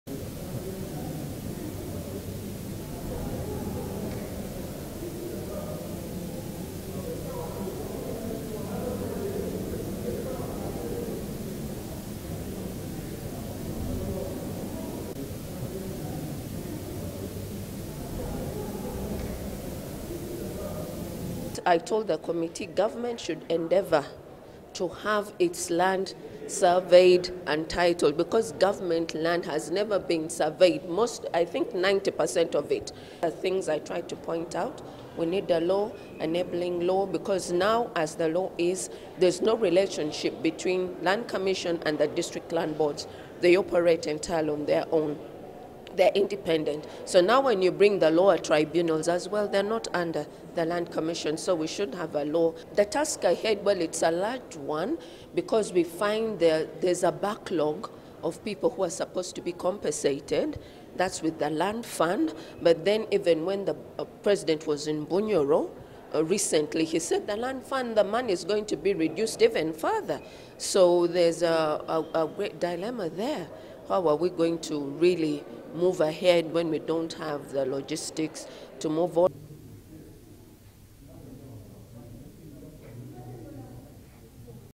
Parliament vets new Uganda Land Commission members
The Parliamentary Appointments Committee chaired by Speaker Rebeca Kadaga has vetted new members of the Uganda Land Commission recently nominated by President Yoweri Museveni. Former Hoima district Woman MP Beatrice Byenkya Nyakaisiki leads the new team.

She is slated to replace Baguma Isoke.

Other nominees include founding Gulu University Vice Chancellor Prof. Pen-Mogi Nyeko who retired recently. Others are former Iganga district LC5 chairman Asuman Kyafu, Stella Achan and former Kabale Town Clerk Rukiika Bujara.